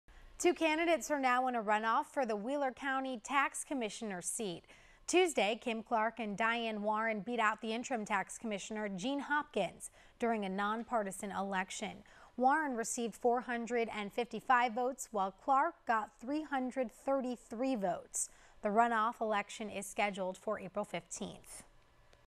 41NBC/WMGT - Wheeler County Tax Commissioner Runoff - 3.19.14
Two candidates are now in a run off for the Wheeler County tax commissioner seat.